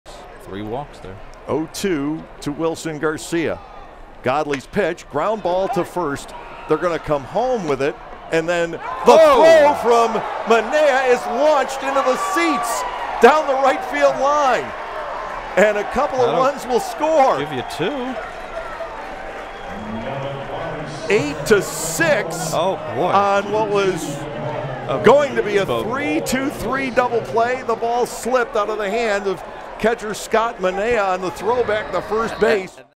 LOOK OUT! Launched into the seats!
Check out this Clip from June 21st Game. Have you seen this before?

Be sure to go follow  @lcrbarnstormers